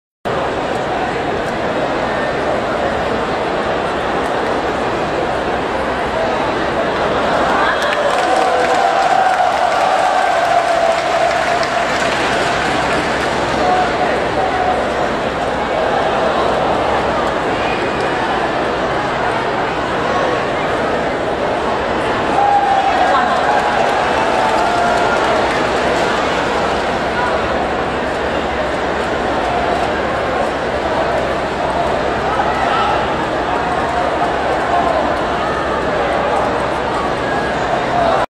Ibrahim Hamato Para Table Tennis Champion
Ibrahim Al Husseini Hamato is an Egyptian Para Table Tennis Champion.